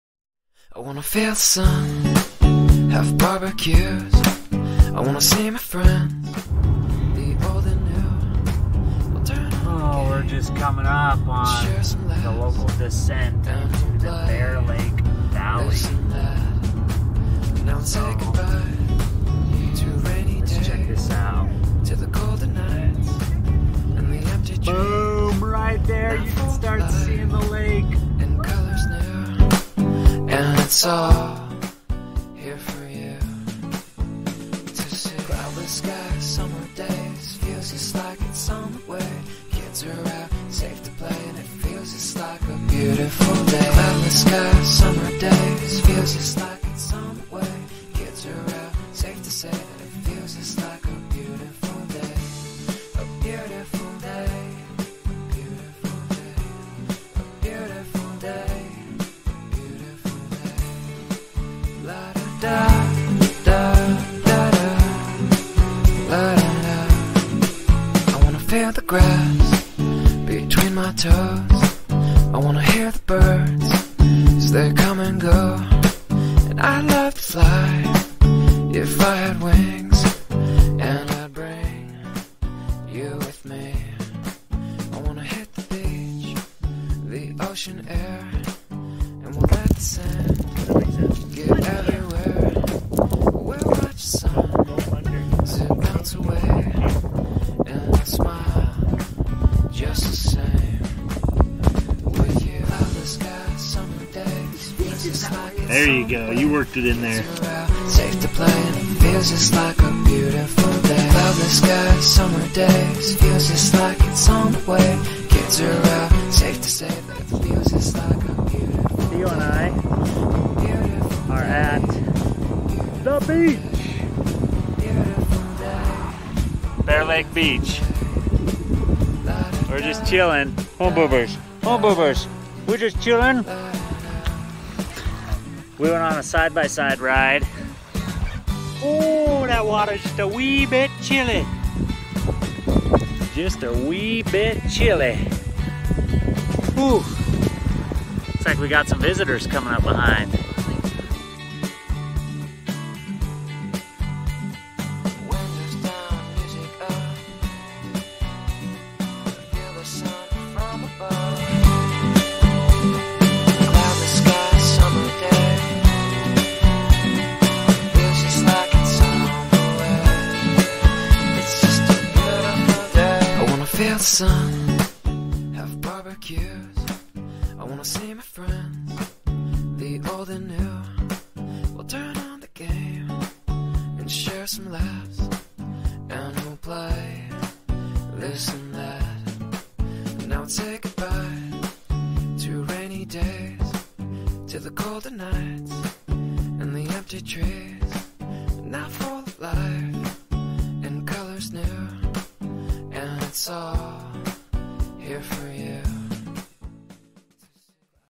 Bear Lake 2018 | A Fall Family Adventure
Join us (the Maceys) and our friends (the Holts) on a fun weekend at Bear Lake on the Utah and Idaho border.

Bear Lake was first discovered to white settlers in 1818 but Native American tribes were there long before that. It's now part of the Utah and Idaho territories and attracts people all year long.

Summers are the most packed, and people wait in line for hours to get famous raspberry shakes in Garden City, Utah.